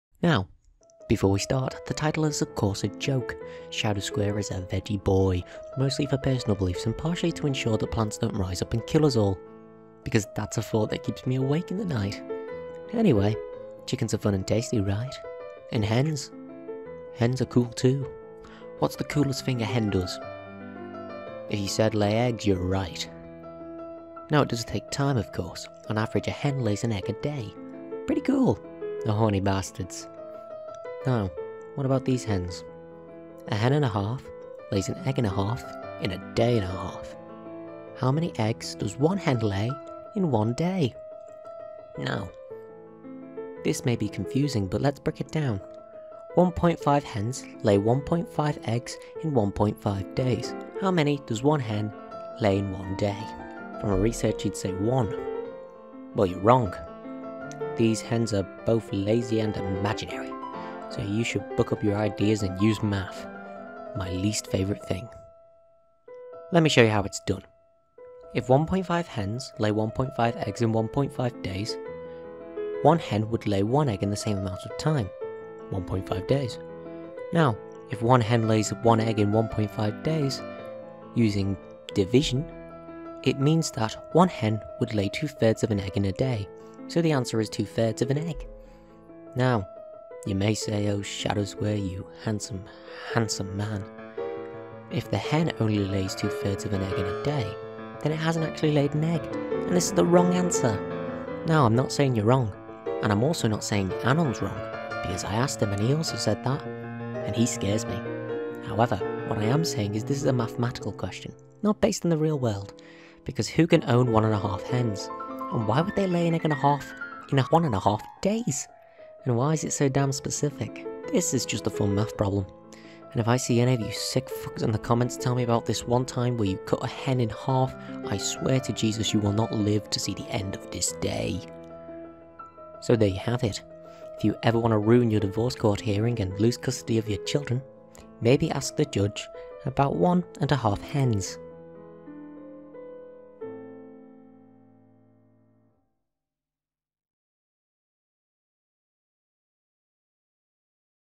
Are you Flummoxed? Episode 3: A Hen Worth Breaking Veganism For
Now chicken is fun and tasty right? And hens are very cool too! What is the coolest thing hens do? You are right, they lay eggs. Now it does take time of course, on average a hen lays an egg once a day, pretty cool right? Now what about these hens?